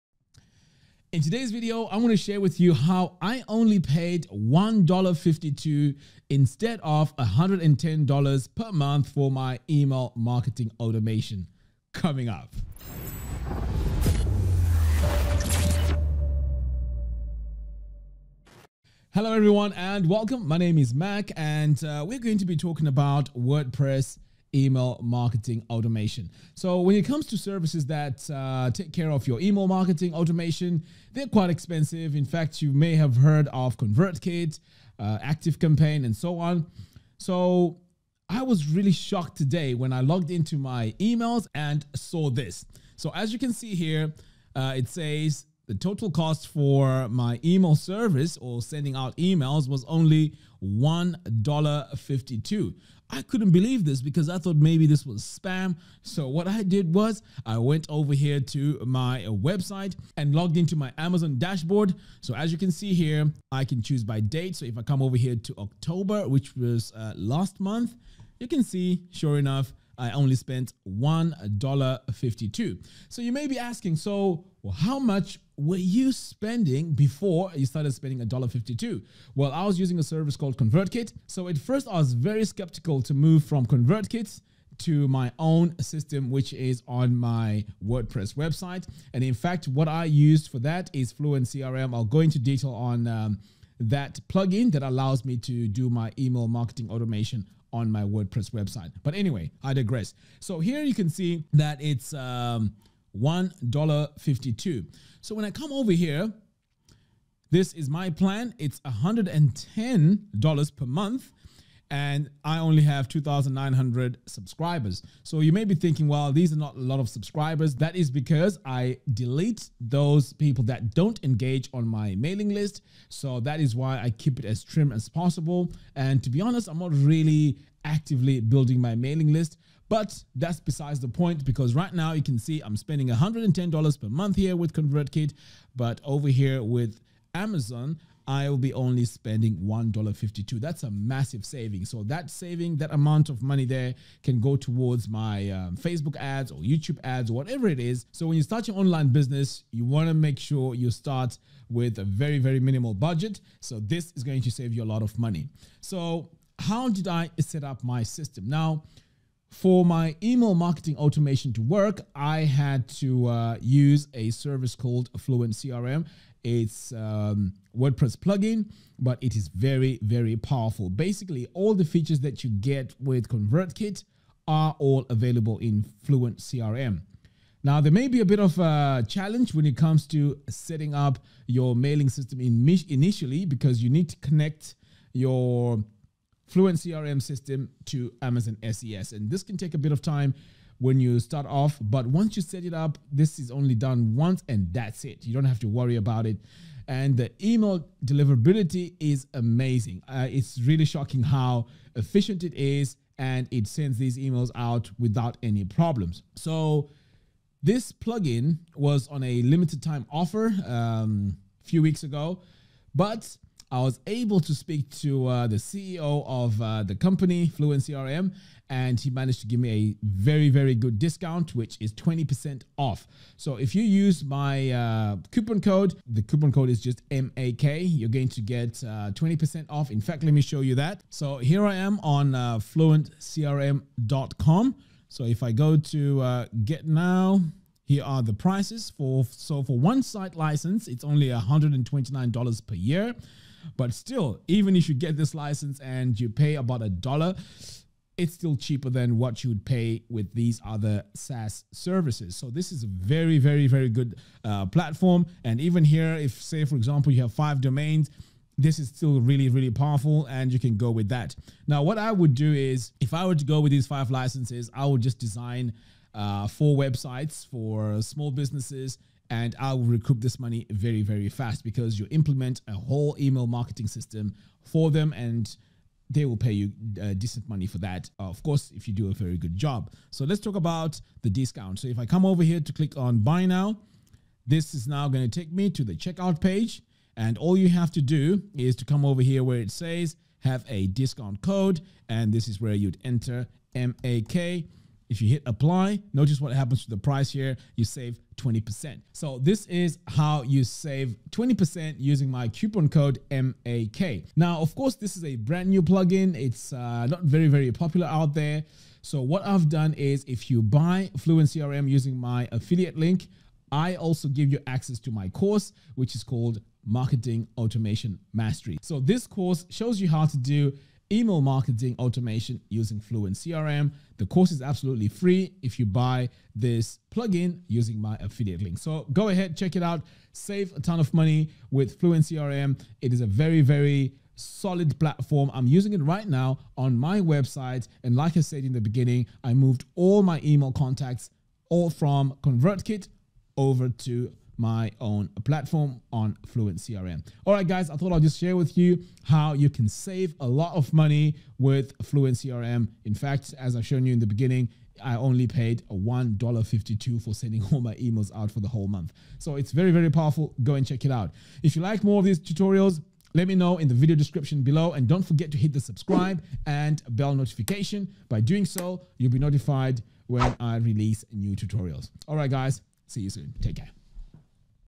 How I only paid $1.52 for email marketing automation using FluentCRM and Amazon SES
In this video, I share how I only paid $1.52 for email marketing automation on WordPress using FluentCRM and Amazon SES.